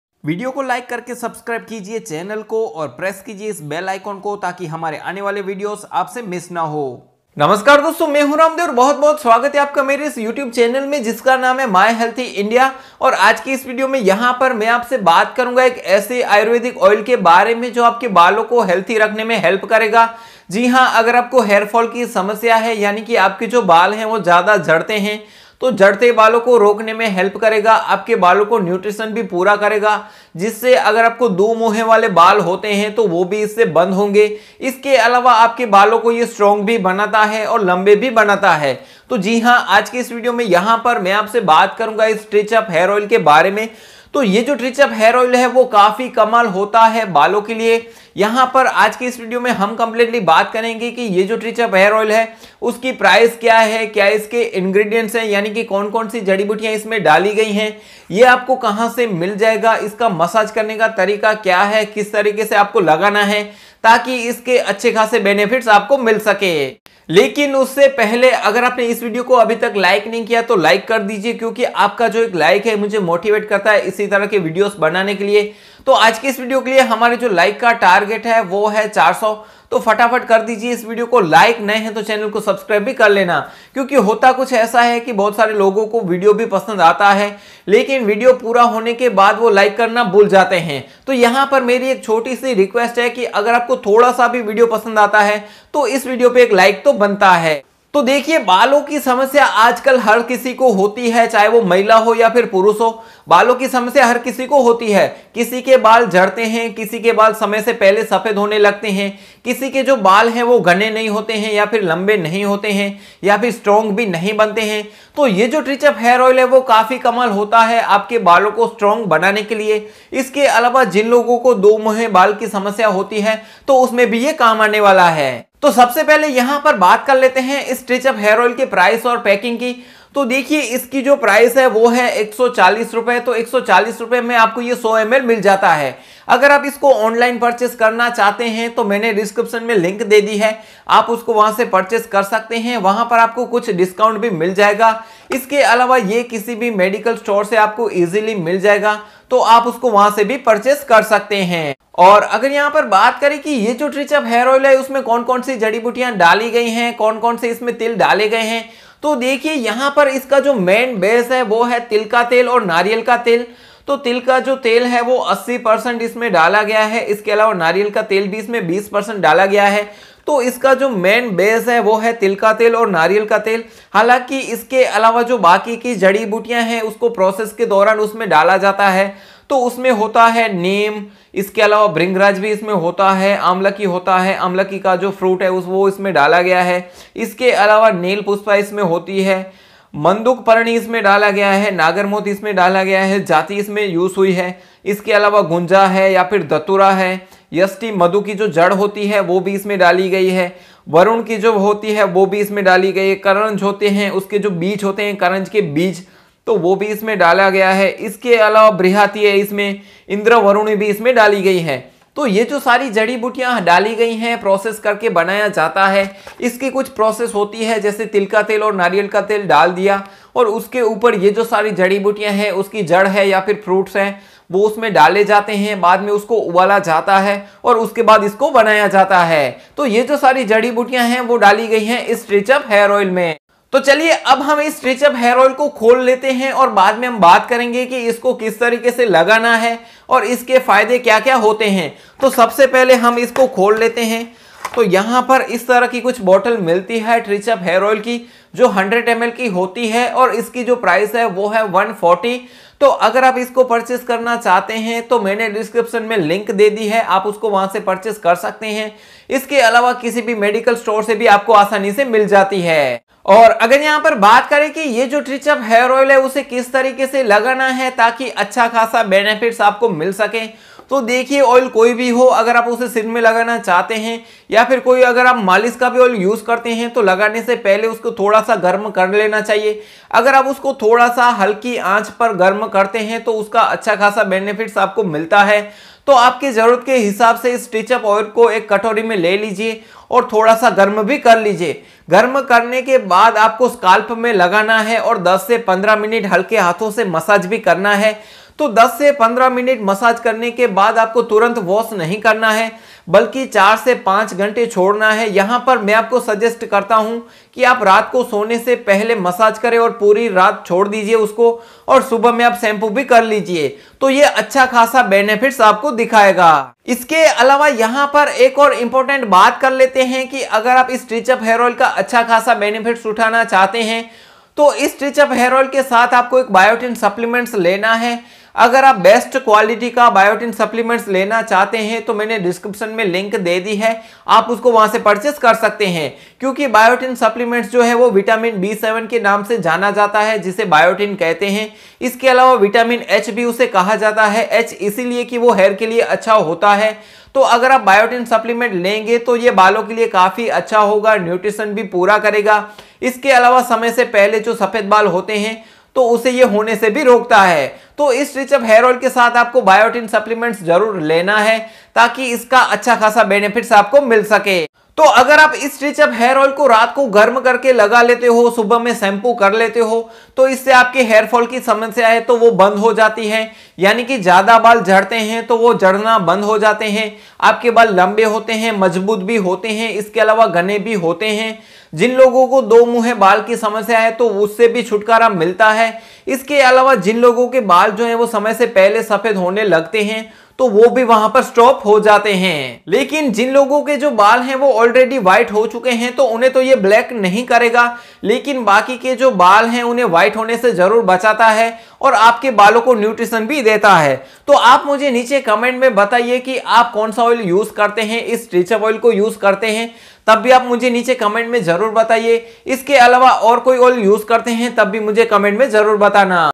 Trichup Hair Oil Review | Stop Hair Fall | Grow Hair Faster
This Video Is About Trichup Hair Oil . Trichup Hair Fall Control Oil is an Ayurvedic product that helpa to prevent premature hair fall and provide nourishment to the inner surface of the cuticles and hair follicles. Trichup Hair Oip contains natural ingredients and herbs that aid hair growth. The oil nourishes your hair with its Ayurvedic ingredients to make it strong and shiny. Watch This video on Trichup Hair Oil for more information.

Trichup Hair Oil

Trichup oil

Biotin Supplement

Biotin Vitamin